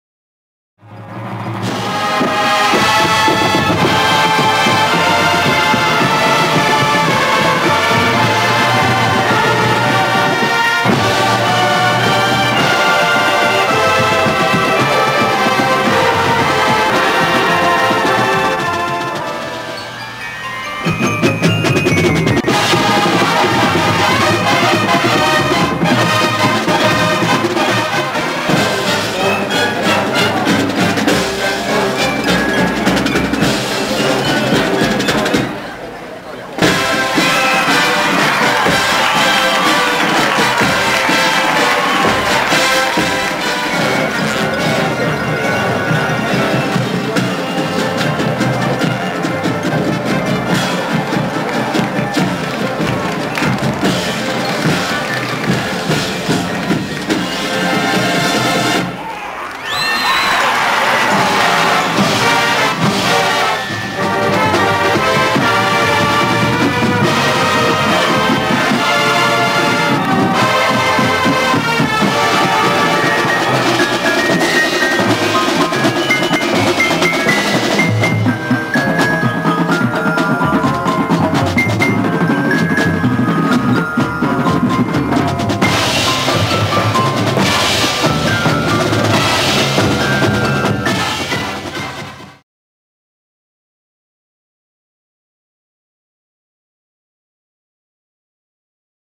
Flushing Marching Raiders 1976-1980 Montage
For a DVD I made for my folks. This is a composite featuring the music from 1979 and 1980 with images from 1976-79. I used it for the menu selection screen.